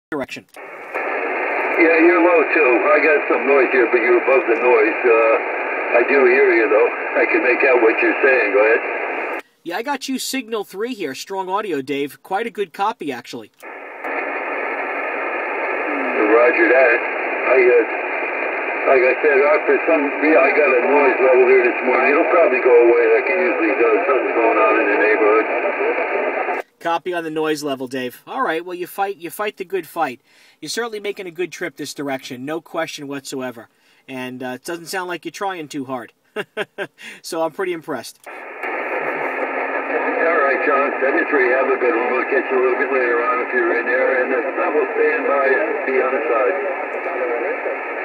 157 Dave NJ Hits New Haven!
157 Dave, Plainfield, NJ makes a nice connection to the New Haven lighthouse.  September 24, 2011.